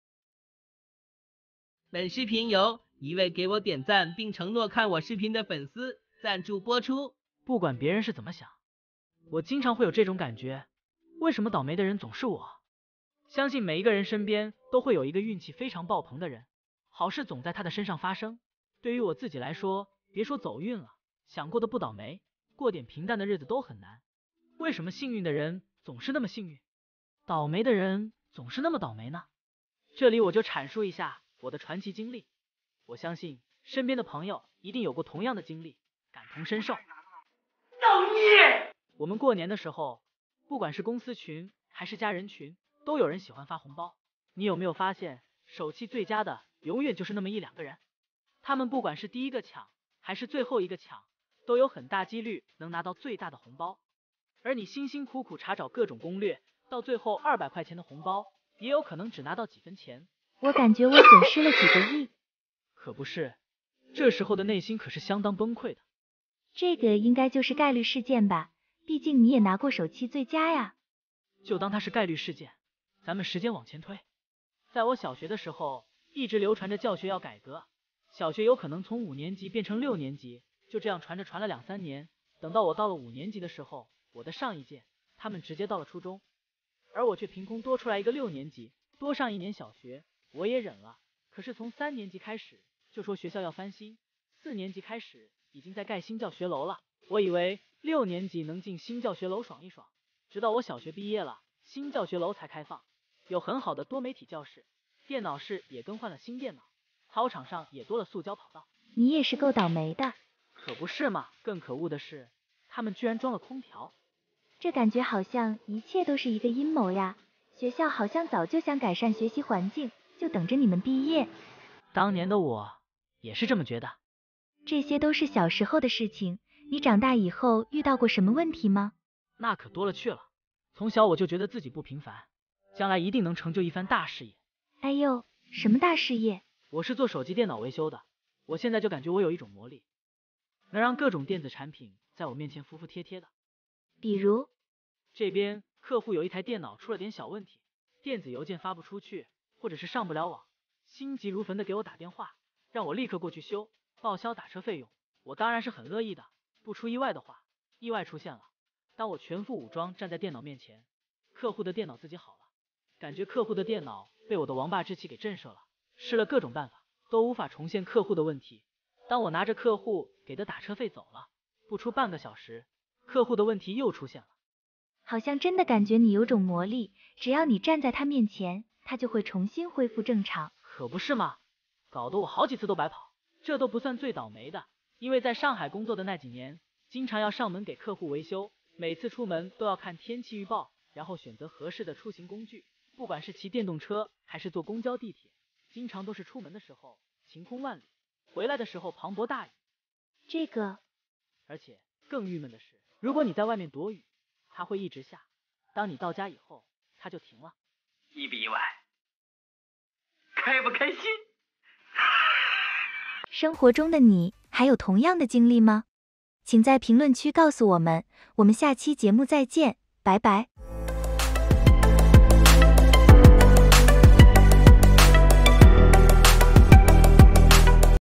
灵魂拷问：为什么倒霉的人总是我？谁能告诉我原因？
古人云：人生不如意十有八九， 若无烦事上心头，便是人间好时节。可是行走在这花花世界中的我，总感觉自己很倒霉，常常蹦出这样的念头